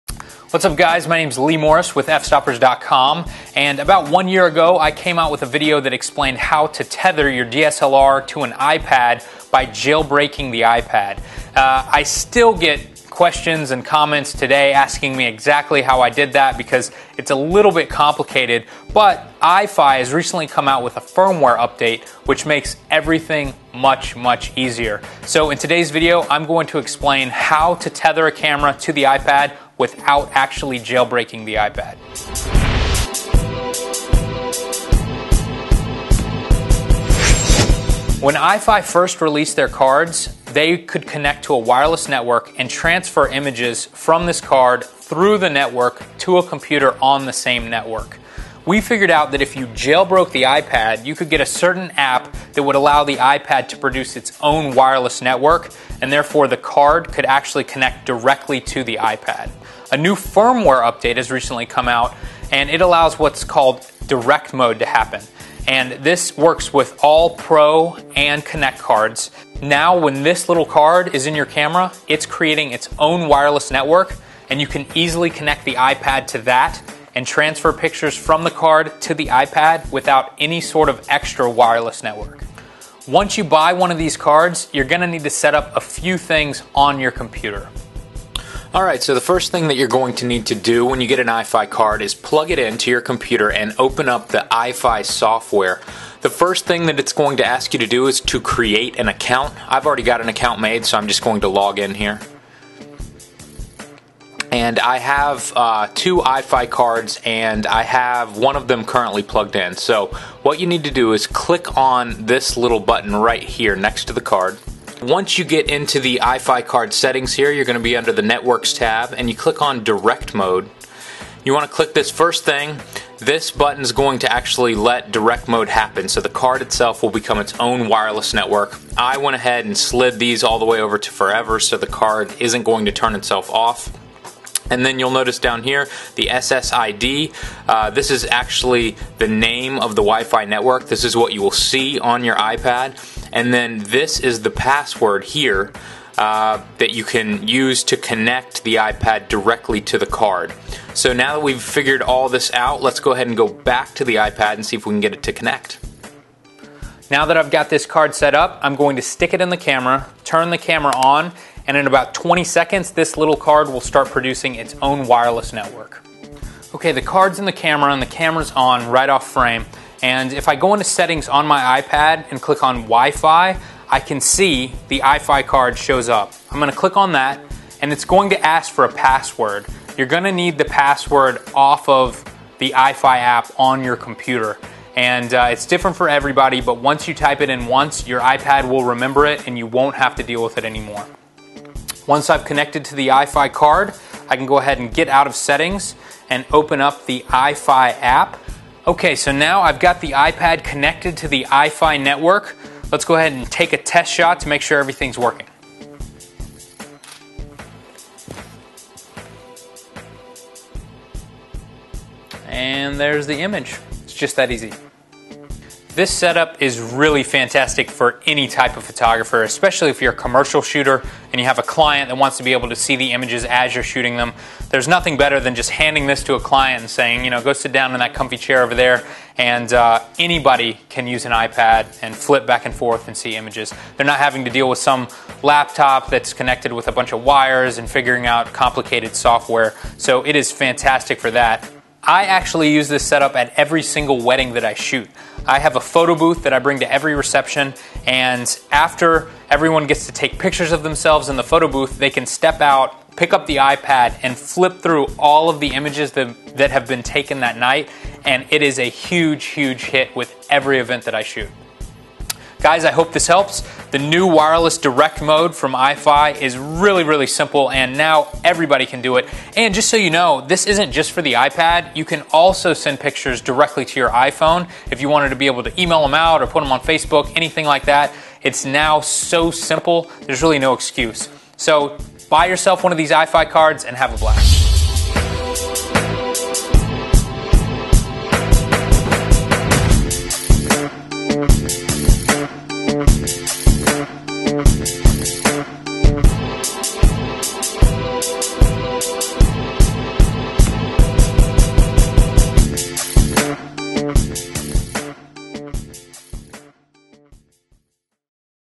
How To Tether Your Camera To An Ipad Without Jailbreaking
All you need is this $40 card

Last year I released a video explaining how to wirelessly tether your camera to an iPad by jailbreaking the iPad.

Since then Eye-Fi has released a firmware update that makes wireless tethering possible without the need for jailbreaking.